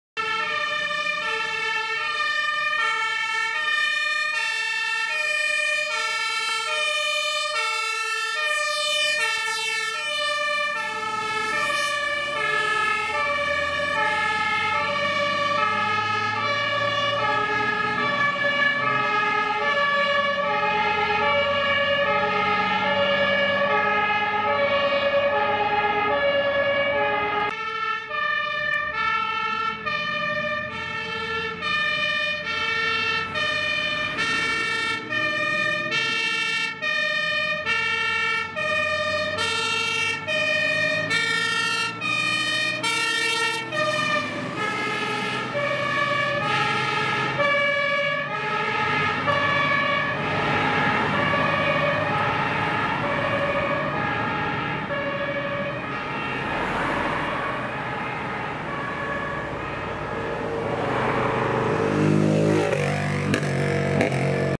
TLF 16/25 + DLK 23/12 FF Greiz
In diesen Video sieht man das Tanklöschfahrzeug 16/25 (FLO GRZ 22) und die Drehleiter mit Korb 23/12 (FLO GRZ 33) der Stützpunktfeuerwehr Greiz.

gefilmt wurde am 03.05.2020